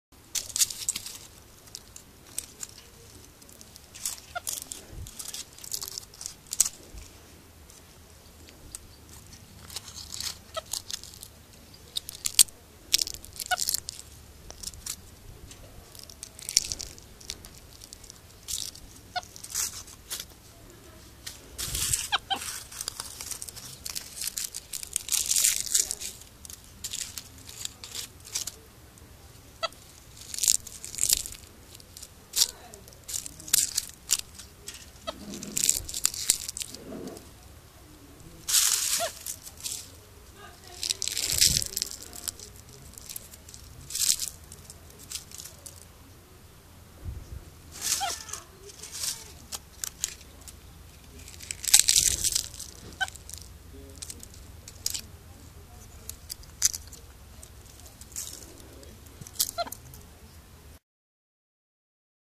International Primate Rescue - Chino Crushing Leaves
This is Chino, a brown capuchin who lives at the International Primate Rescue Centre in South Africa, teaching me to crush leaves! Visit their website at iprescue.org to see all the amazing work they do!

Louise x